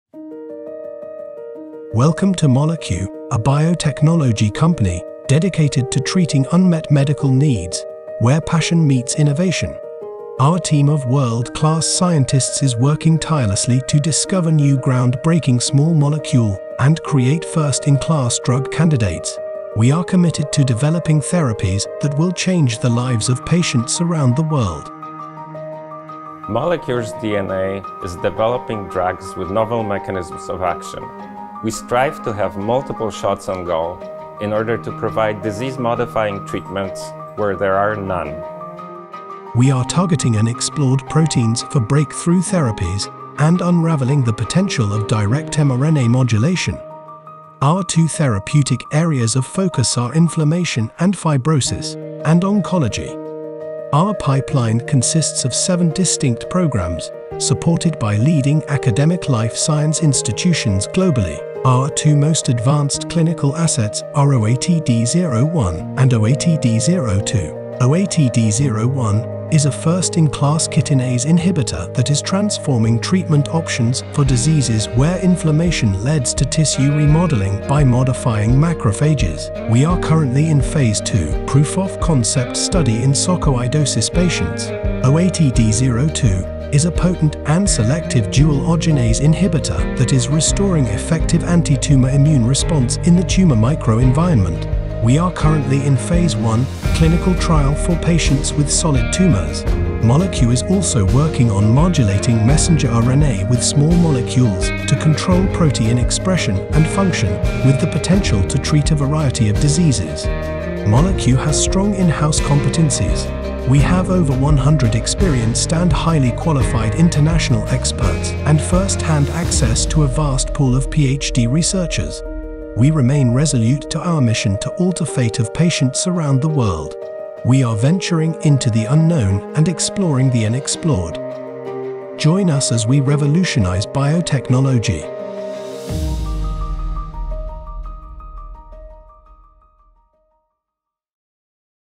About Molecure 2023 - PL subtitles
Molecure is a clinical stage biotechnology company that uses its world leading medicinal chemistry and biology capabilities to discover and develop first-in-class small molecule drug candidates that directly modulate underexplored protein targets and the function of RNA to treat multiple incurable diseases.

Our extensive in-house expertise has allowed us to collaborate with top global academic research institutes to access untapped, novel biology targets.

We have generated a diverse pipeline of seven distinct programs with the support of leading academic life science institutions globally, including Yale University, Rutgers University, the Flemish Institute for Biotechnology (VIB) in Ghent, the University of Michigan and the International Institute of Molecular and Cell Biology in Warsaw (IIMCB), which has significant expertise in RNA science and technology.

Our most advanced in-house drug candidate is OATD-01, a first-in-class dual chitinase inhibitor for the treatment of interstitial lung diseases, such as sarcoidosis and idiopathic pulmonary fibrosis, that is Phase II ready. A Phase II trial in patients with sarcoidosis is expected to start in 2023.

Our second proprietary candidate is OATD-02, an oral, potent and selective first-in-class, dual arginase inhibitor (ARG1 and ARG2) for the treatment of cancer, which is at the stage of Phase I clinical development.

Molecure’s headquarters and laboratories are located in Warsaw, with an additional laboratory facility in Łódź, Poland. The company is listed on the Warsaw Stock Exchange (ticker: MOC).